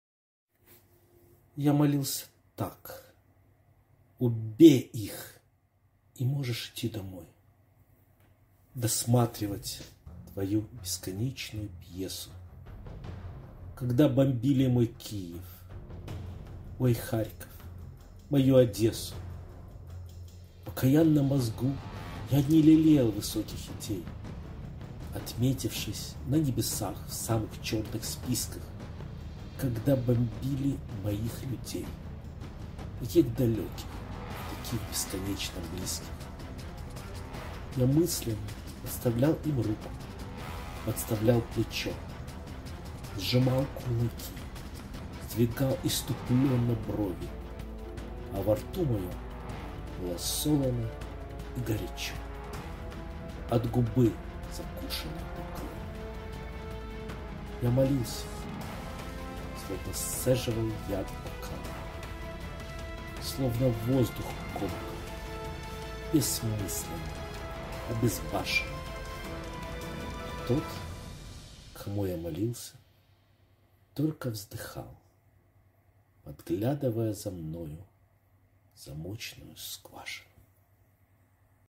Молитва – Михаил Юдовский
Использованы фотоматериалы из Сети, связанные с немецко-фашисткой агрессией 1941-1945 годов и российско-фашистской агрессией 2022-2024 годов.

Текст

Я молился так: "Уб*й их - и можешь идти домой
досматривать твою бесконечную пьесу",
когда бомбили мой Киев, мой
Харьков, мою Одессу.

В окаянном мозгу я не лелеял высоких идей,
отметившись на небесах в самых черных списках,
когда бомбили моих людей -
таких далёких и таких бесконечно близких.

Я мысленно подставлял им руку, подставлял плечо,
сжимал кулаки, сдвигал исступлённо брови.
А во рту моем было солоно и горячо -
от губы, закушенной до крови.

Я молился - словно сцеживал яд в бокал,
словно воздух комкал - бессмысленно, обезбашенно.
А тот, кому я молился - только вздыхал,
подглядывая за мною в замочную скважину.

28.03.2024